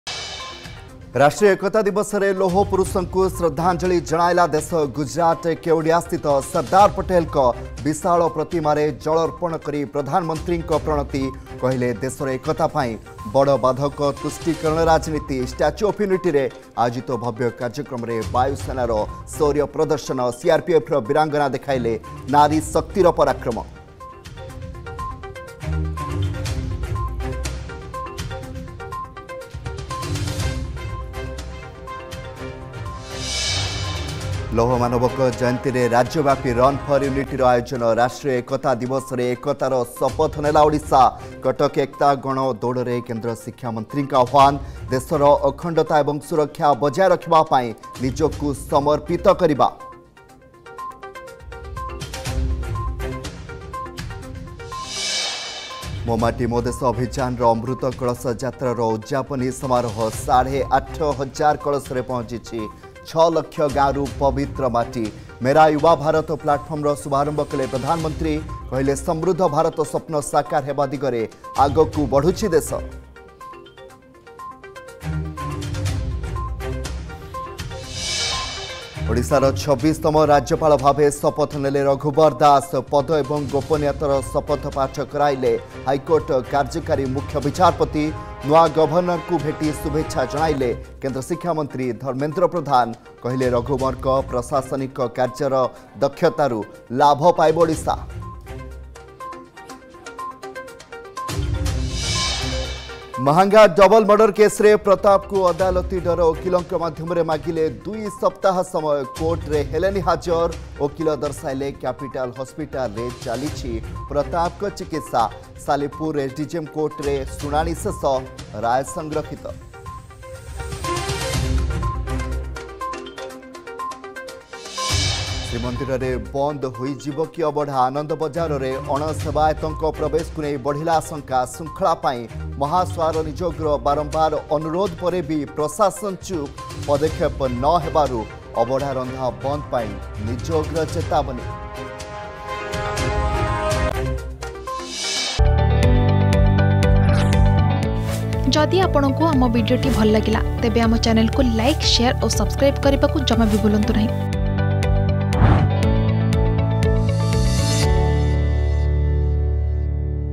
Headlines @ 7 Pm | 31 Oct 2023 | Argus News
+ ଲୌହ ପୁରୁଷଙ୍କୁ ଶ୍ରଦ୍ଧାଞ୍ଜଳି
+ ଏକତା ପାଇଁ ଦୌଡ଼ିଲା ଓଡ଼ିଶା
+ ମୋଦିଙ୍କ ମୁଣ୍ଡରେ ମାଟିର ଟିକା
+ ଶପଥ ନେଲେ ରଘୁବର